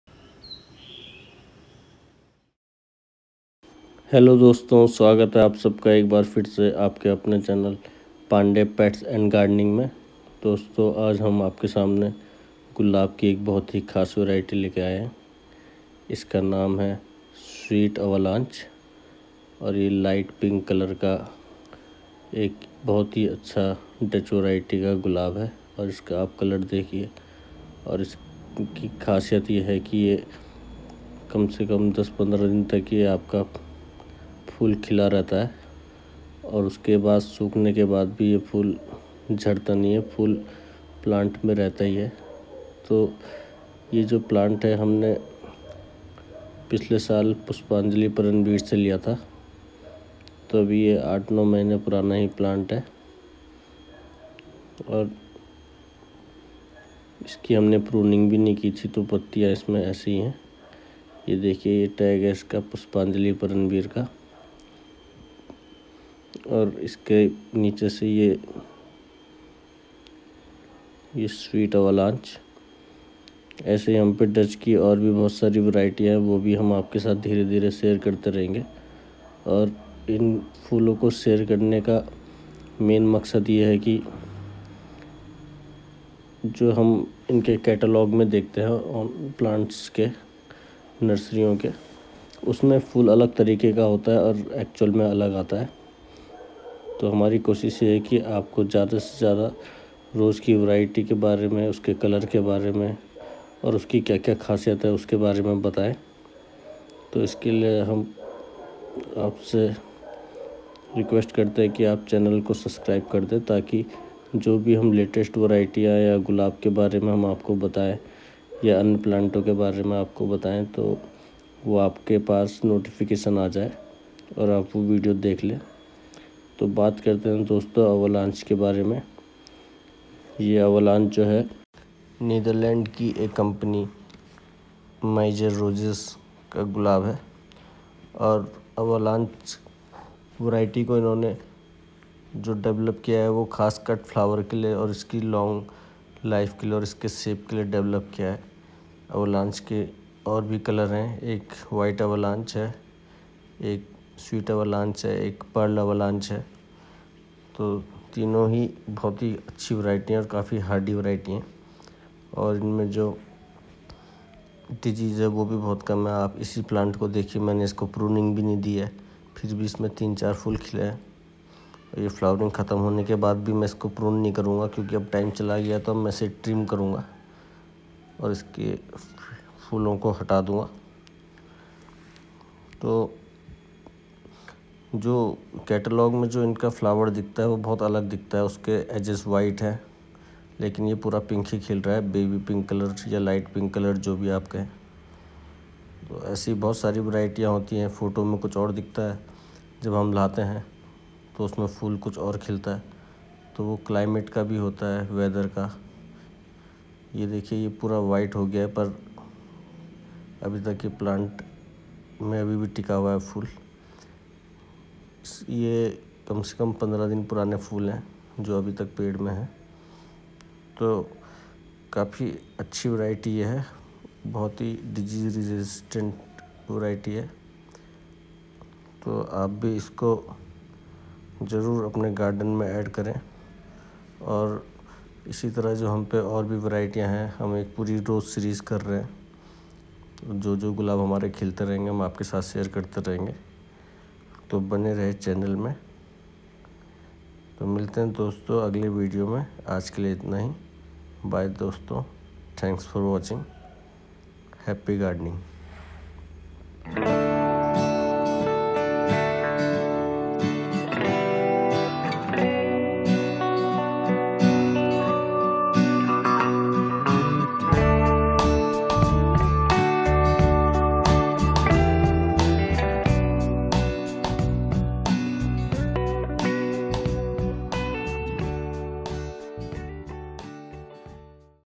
SWEET AVALANCHE ROSE
Hello everyone in this video we will tell you about a rose verity sweet avalanche it is a dutch rose use in cut flower. we also orking on our other rose verity so kindly watch the full video and make sure to subscribe us.